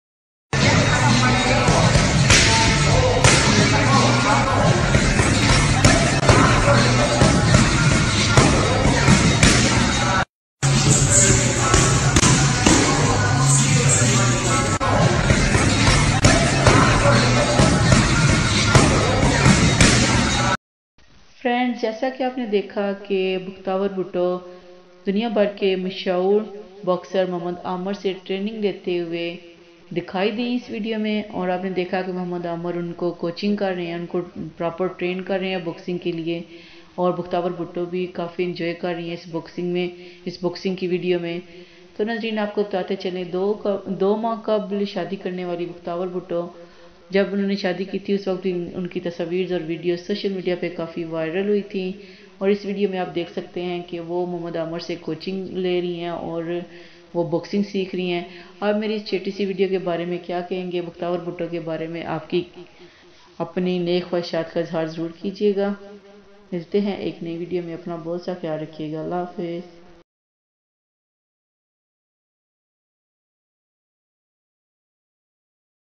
Bakhtawar Bhutto Zardari New Video Viral || Bakhtawar Bhutto Boxing Video with Mohammed Amir
Bakhtawar Bhutto Boxing Training With Amir Khan

Boxer Amir Khan on Sunday met with Aseefa and Bakhtawar, the daughters of former Prime Minister Benazir Bhutto and former President Asif Ali Zardari in Dubai and shared boxing tips with them.
Speaking exclusively to Private TV Channel from Dubai, Amir said he had a really great time with them, further adding
that the duo also invited him to visit Lahore.
“I met Bakhtawar and Aseefa through my wife Faryal and they invited me to Lahore. We had some food with them and had a great time with them. They are really really nice people, [their] hospitality was amazing.,” Amir said.
The former world champion was all praises for Bakhtawar Bhutto, he said that he had a boxing training session with Bakhtawar Bhutto.
“We went to the gym; as Bakhtawar trains and she loves boxing so I trained her with the coach, helped her. We learnt boxing and everything. She did great. Honestly, she boxes very good. She is very strong. She is very committed and very focused.”
“Bakhtawar loves boxing and I trained with her in the gym,” he said, adding he also said that she gifted him and his wife the autobiography of her mother, “Daughter of Destiny”. The former world champion also shared videos of him training with Bakhtawar in the gym, with Bakhtawar showing her punching skills during the training session.

Bakhtawar Bhutto First video after wedding
Bakhtawar bhutto and Mohammed Amir boxing training
Bakhtawar bhutto husband Chaudhry Mehmood